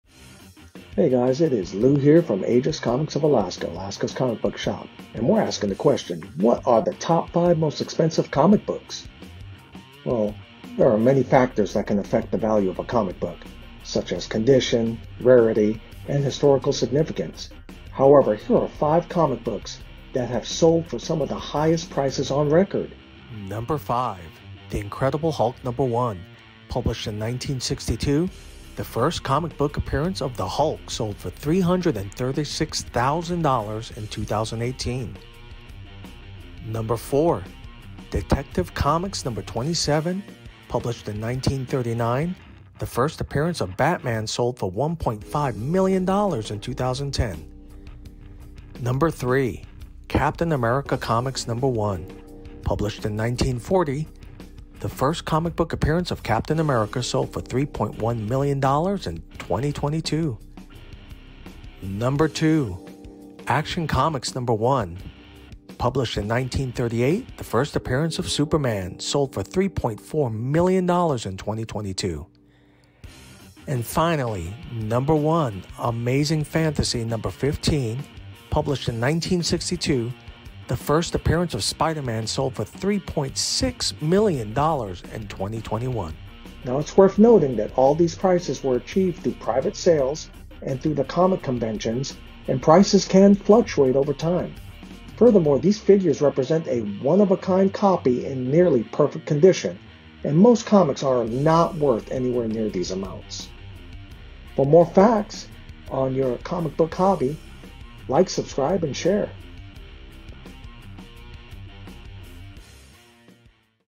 Top 5 Most Expensive Comic Books
The placement of the top 2 spots may surprise you!

📺  OUR EQUIPMENT LINKS:

🎙 Sound System
🎥. Ecamm Software-